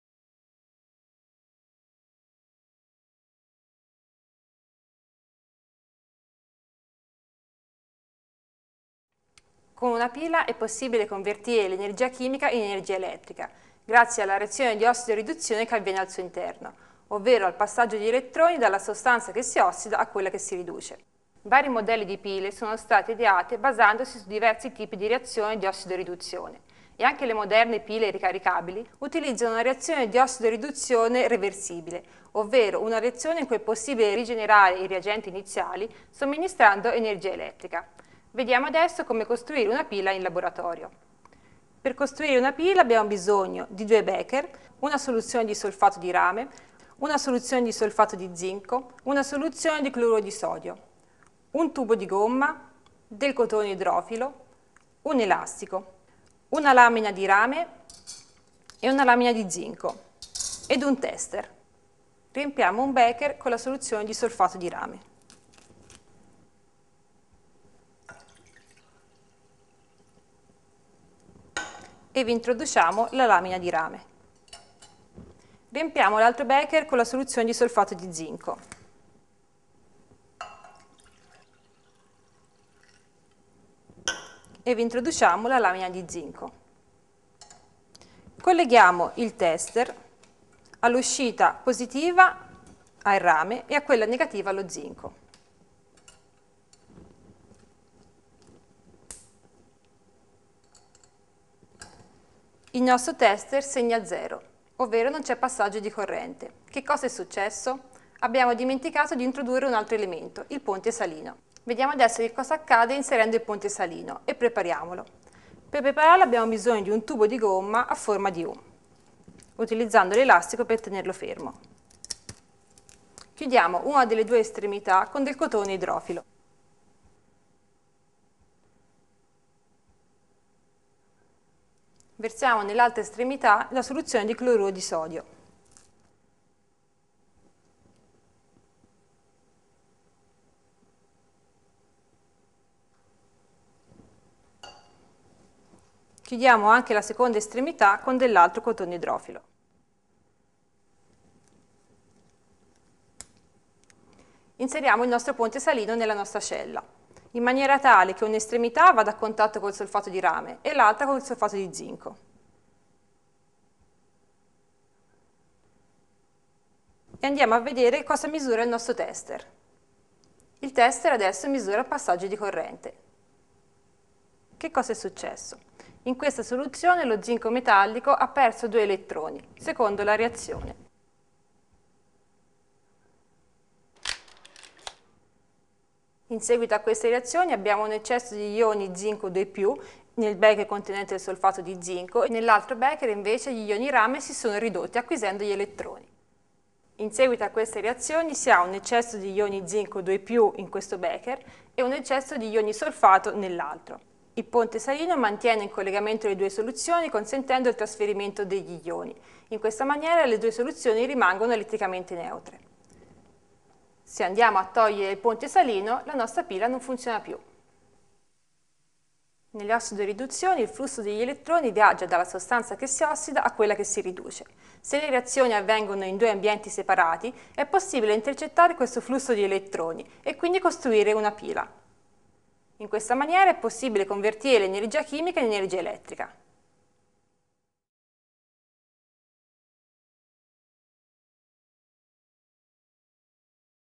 Idee della chimica - La pila Daniell
Idee della chimica (Valitutti, Tifi, Gentile) seconda edizione - Zanichelli

La pila Daniell  Facciamo l'energia